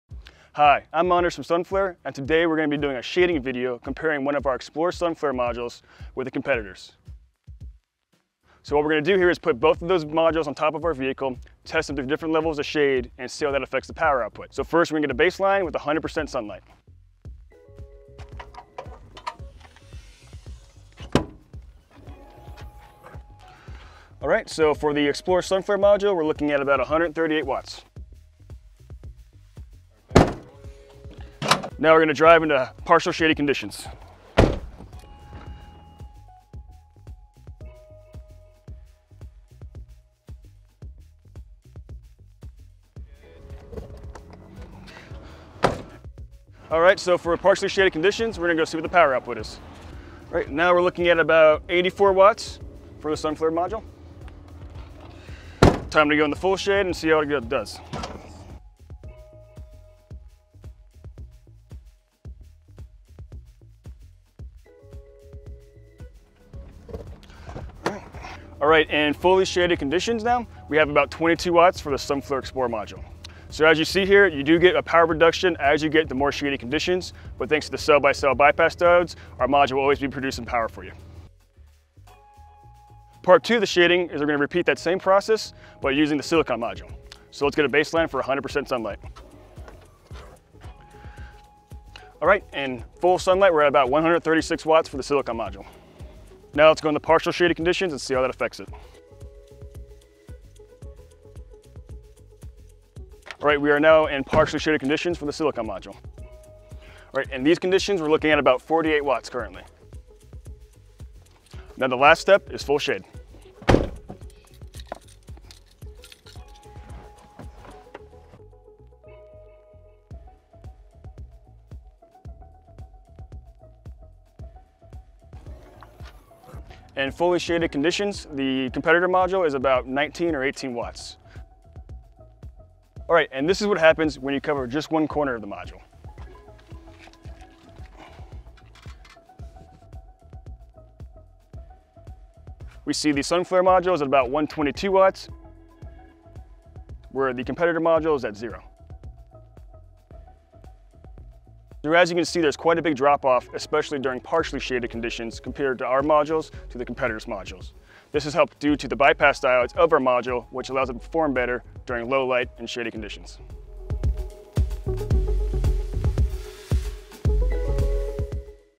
XPLOR Solar Panels vs. Shade
For most solar panels, the enemy is shade. It can shut down the whole panel very easily. That’s why XPLOR have added a bypass diode into each cell.  That way, only the cells that are shaded turn off.  With most silicon panels there is a bypass diode per string or sometimes 1 diode for 2 strings. This video demonstrates how shading affects XPLOR solar panels.